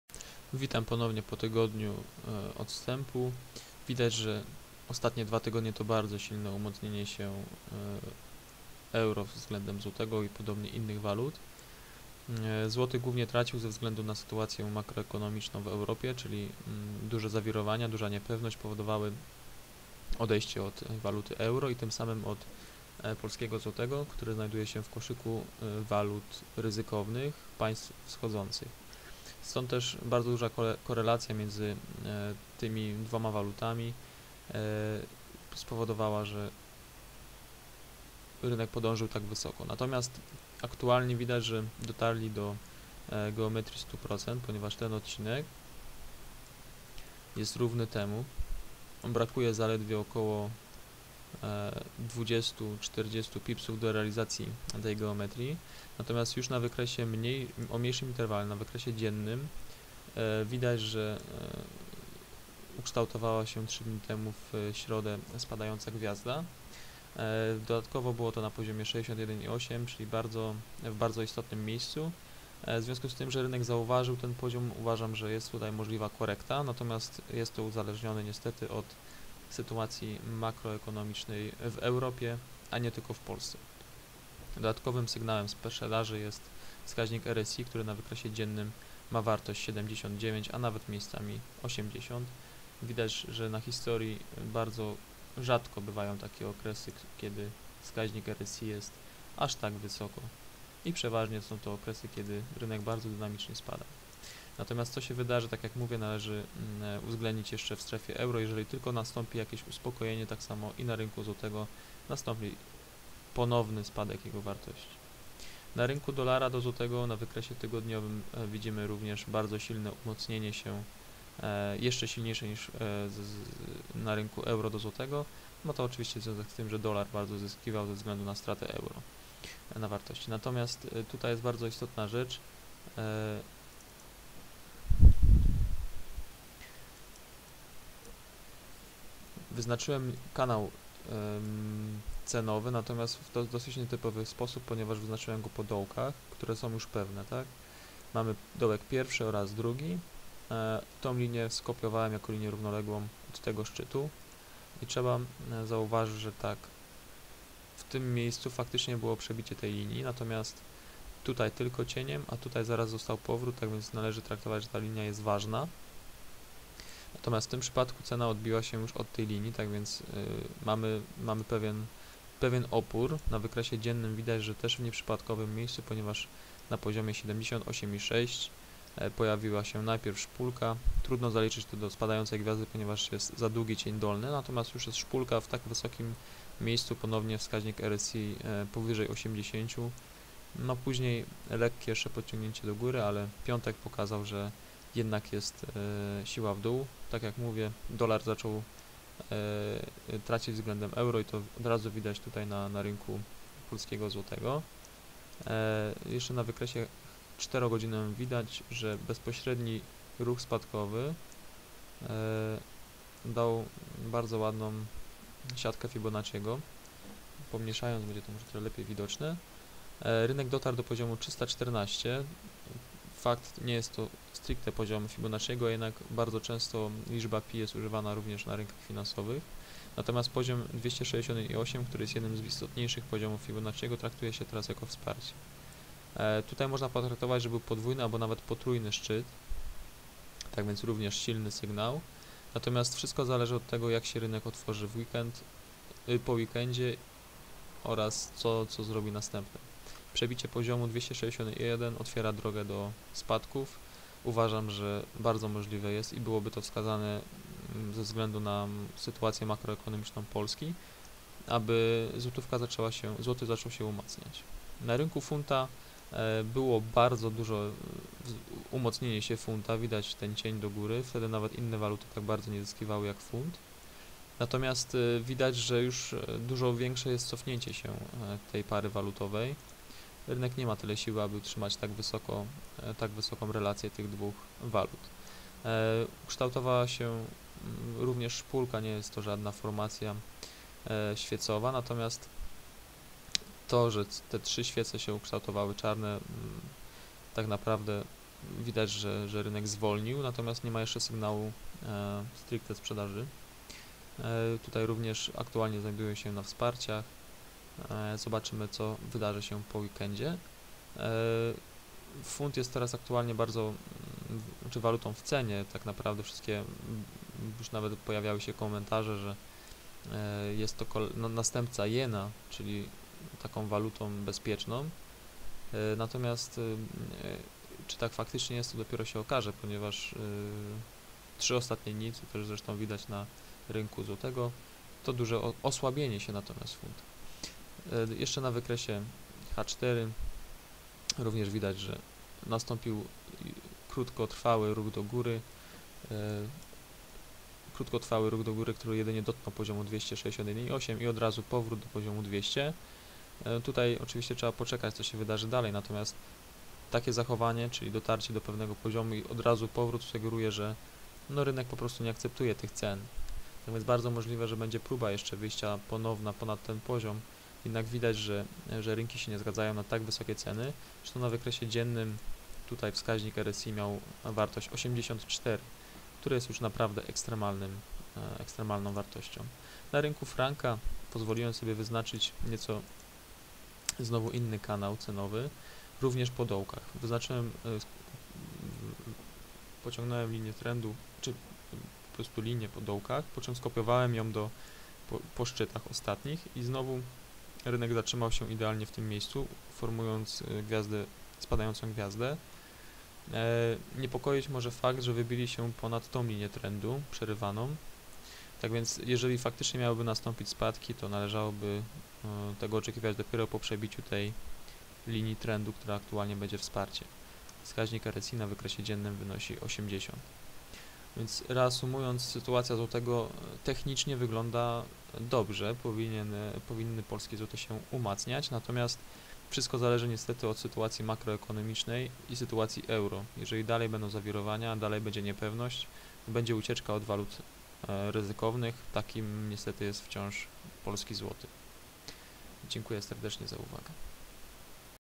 Sytuacja na polskim złotym -- EUR/PLN, USD/PLN, GBP/PLN, CHF/PLN, 20.05.2012
Wideoanaliza sytuacji technicznej na rynku PLN, po dwóch tygodniach bardzo silnego osłabienia się złotego rynki dały sygnały spadkowe.